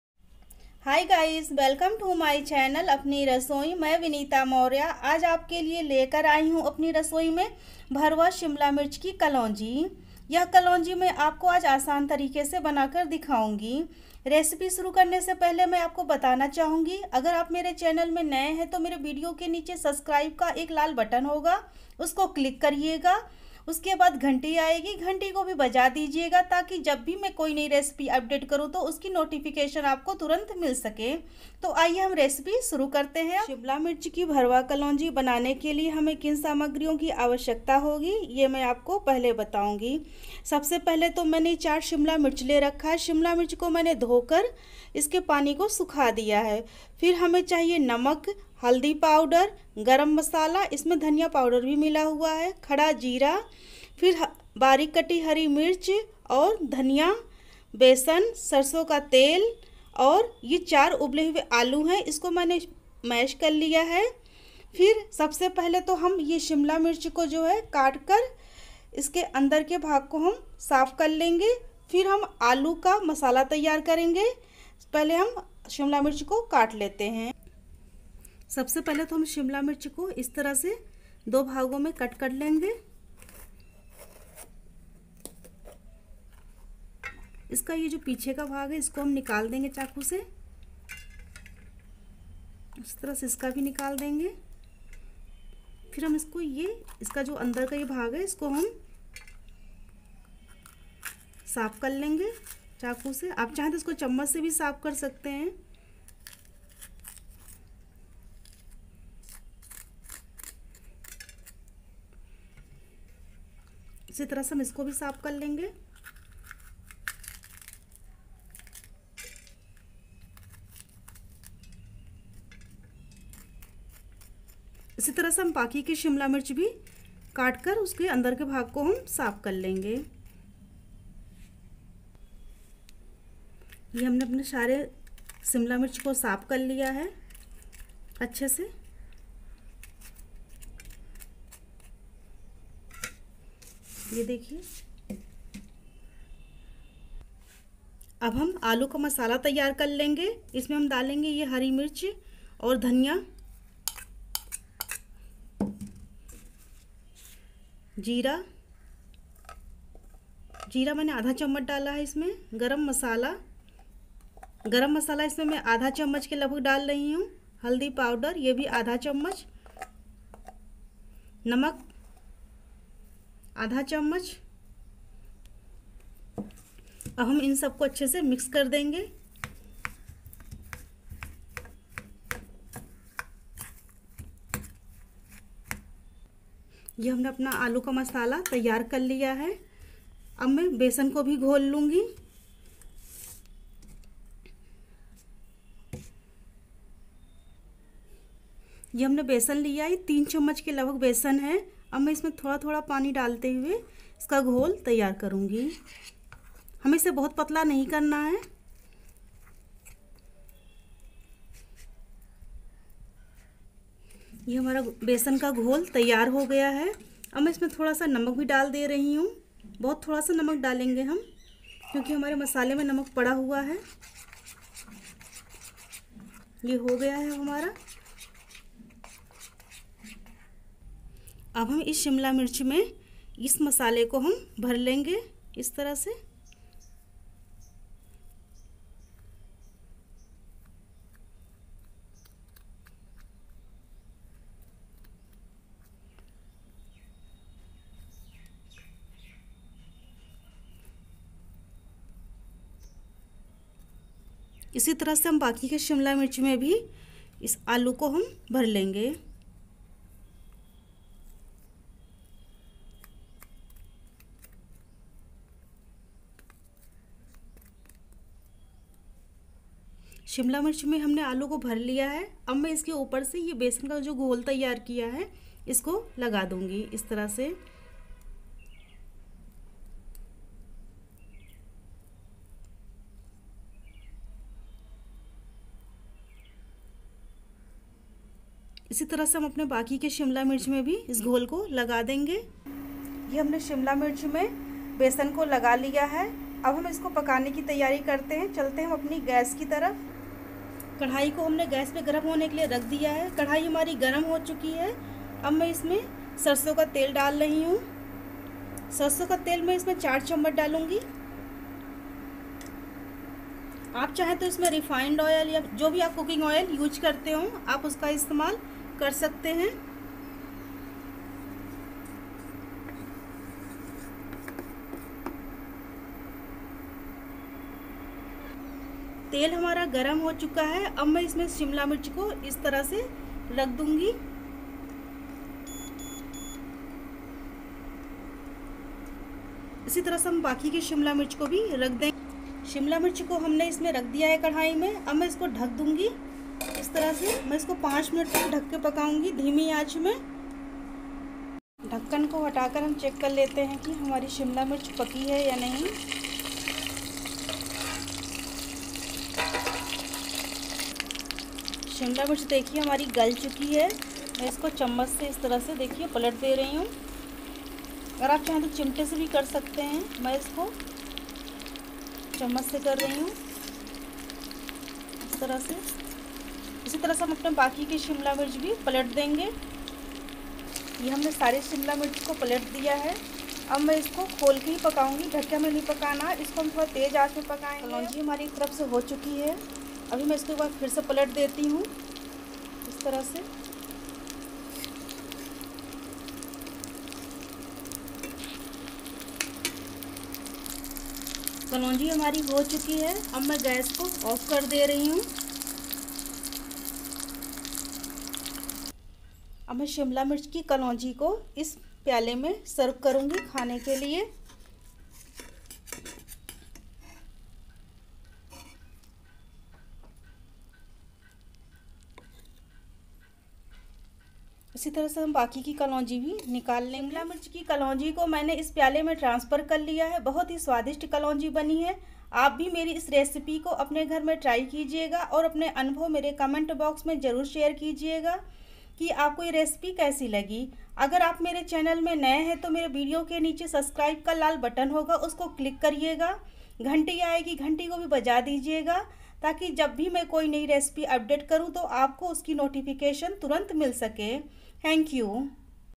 नये और आसान तरीके से बनाये भरवा शिमला मिर्च की कलौंजी।shimla mirch recipe. Stuffed capsicun in hindi
इतनी टेस्टी भरवा शिमला मिर्च की उँगलिया चाटते रह जाओगे।

My channel click that Bell🔔 icon for more notifications.